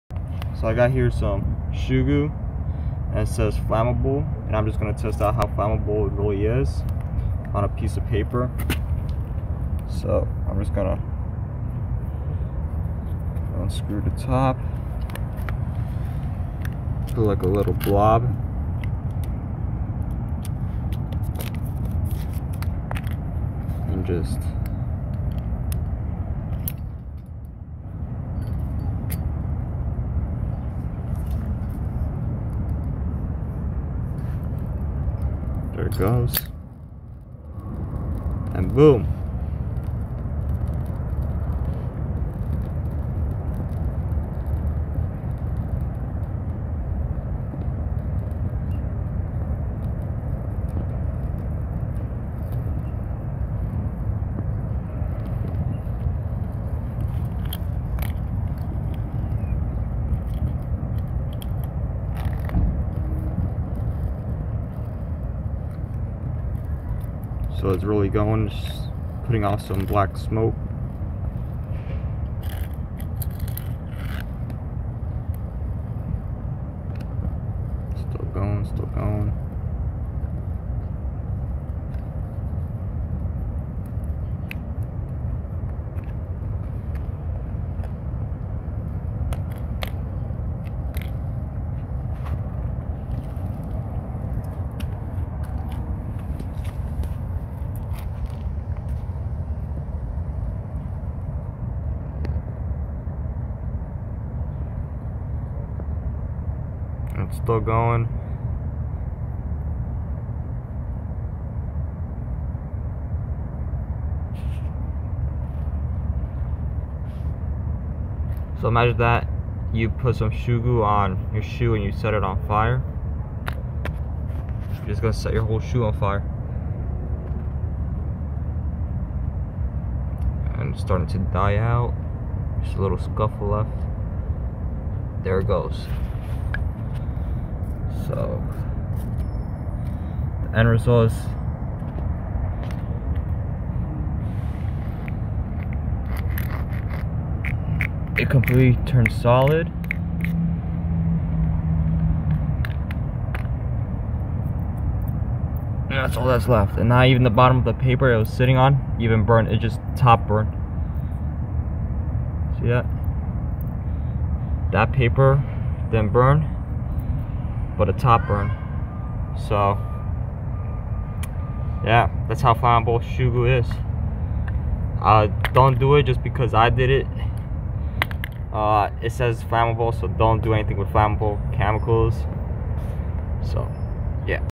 How flammable is shoe goo?
Testing out the flammablility of shoe goo and if it will actually burn